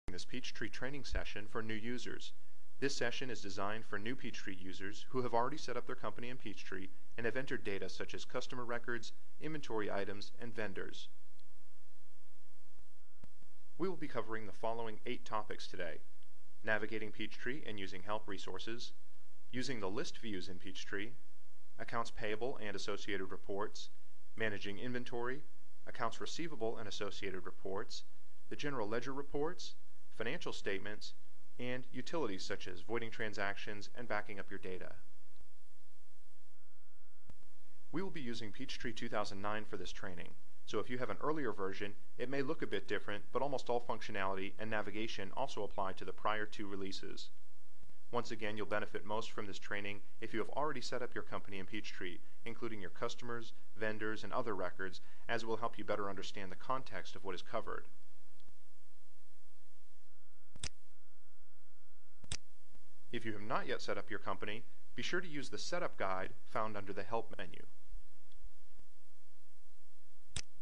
Sage Peachtree Accounting Tutorial: Basics for New Users
for more information on Sage Peachtree.

For more information or questions about Sage Peachtree Support and Training please

Basics for New Users is a Sage Peachtree Tutorial aimed at new users trying to get acclimated with Sage Peachtree accounting. This training session will cover product features such as navigation, accounts payable, inventory accounts receivable, ledger reports and more.

Was this video helpful to you? Let us know by leaving a comment!